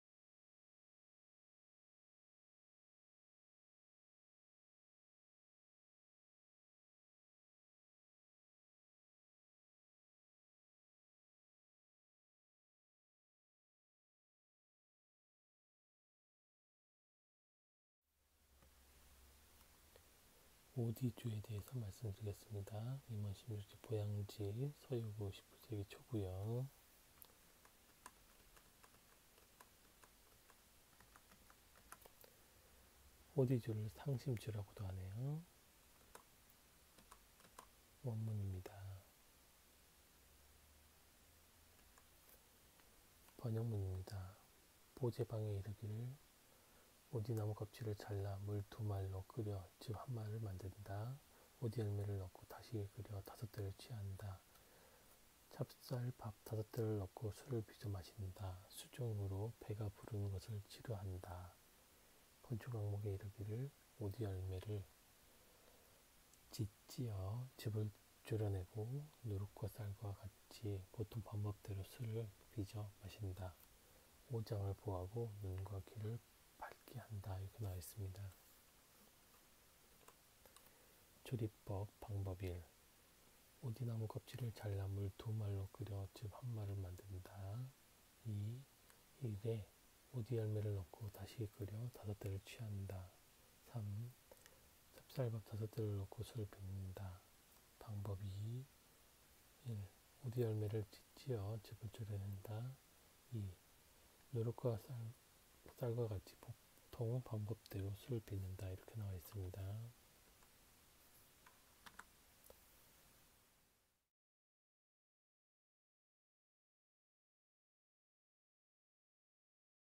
한국 전통주(Korean Alcoholic Drinks) 160. 오디주 - 임원십육지-보양지
1. 전통술 커버이미지 : Korea Traditional liquor로 표시함.
  2. 이미 묶음 영상으로 올렸던 전통술 제목에는 Korea Traditional Sake로 표기함.
  3. 이번에 올리는 각각의 전통술에 대해서는 Korean Alcoholic Drinks로 표기합니다. 범위가 더 넓은 의미로 한국 알콜음료로 적용했습니다.

- 7인치 이상의 태블릿, 노트북, PC 모니터에서 시청하시길 추천드립니다.